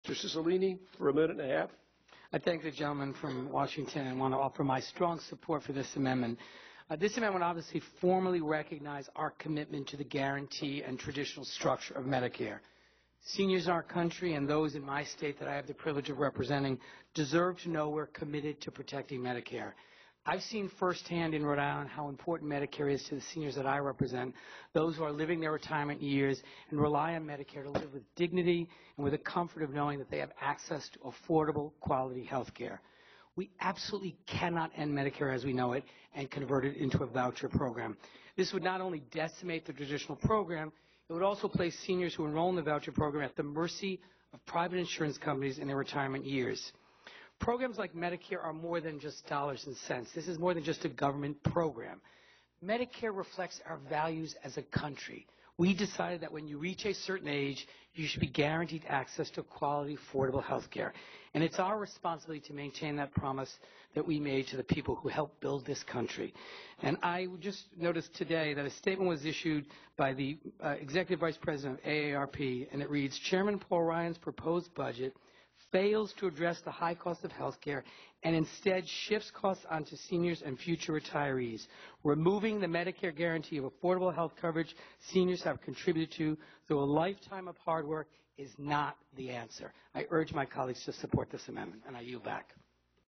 Ryan Budget Markup - Cicilline on Medicare
U.S. Congressman David N. Cicilline (D-RI) emphasizes the importance of protecting Medicare during House Budget Committee consideration of the Ryan budget.